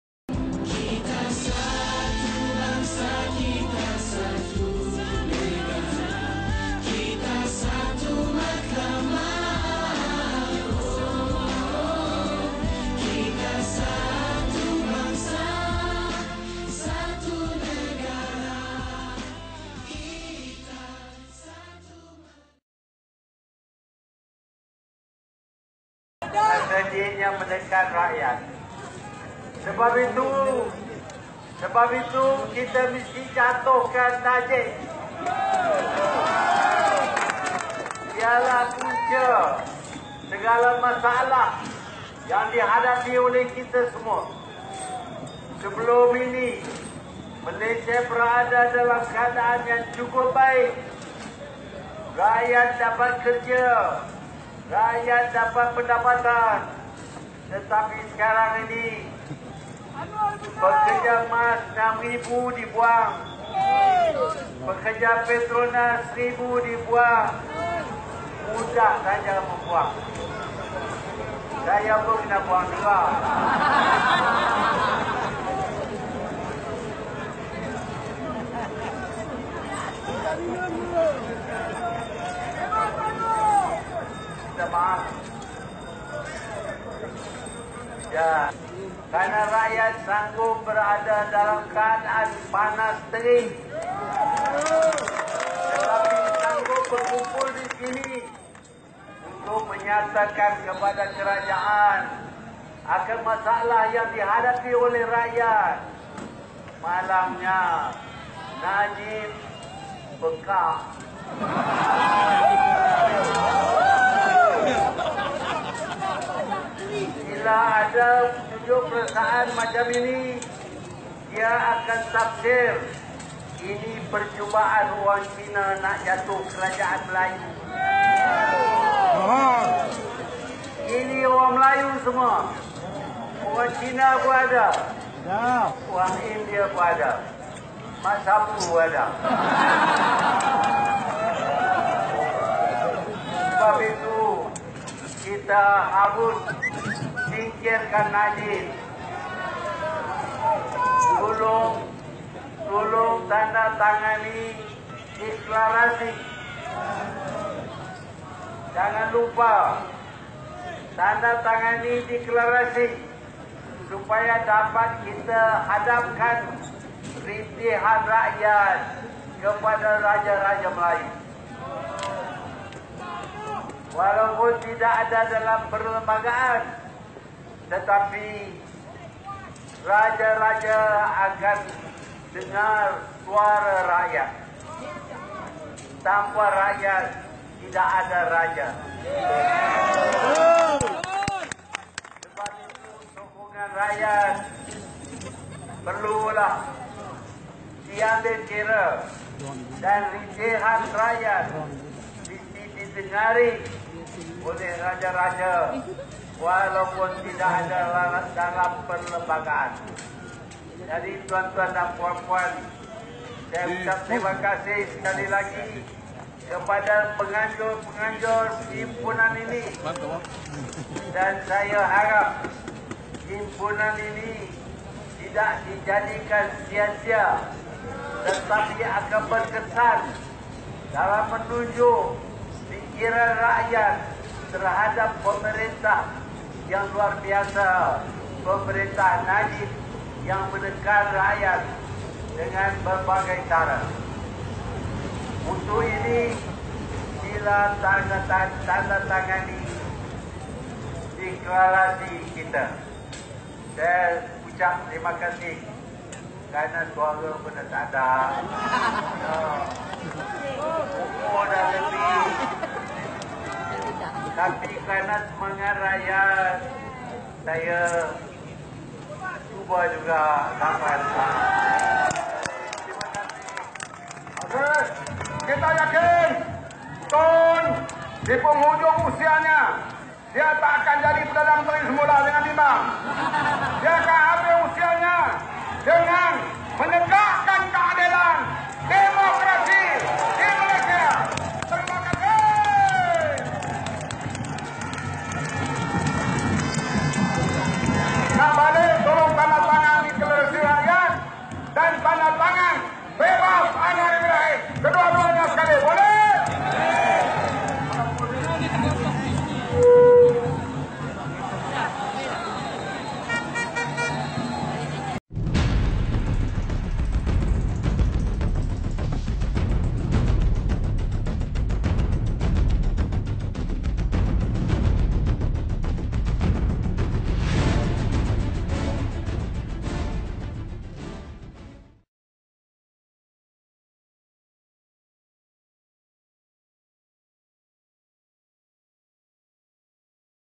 Bekas PM Tun Dr Mahathir turut hadir ke himpunan anti GST. Hari ini dalam sejarah.
Tun Dr Mahathir minta rakyat jangan lagi ditipu  Negara hampir muflis & skandal 1 MDB telah jatuhkan maruah & imej negara.
Bekas PM Tun Dr Mahathir turut hadir ke himpunan anti GST di dataran Merdeka. Hari ini dalam sejarah.
Tun Dr Mahathir ceramah atas lori minta Rakyat TOLAK kerajaan BN yang menekan hidup rakyat. GST telah membawa susah kepada semua.
Tun Dr Mahathir kata skandal 1 MDB sudah jadi mala petaka besar kepada negara. Rakyat perlu bangkit untuk selamatkan Malaysia!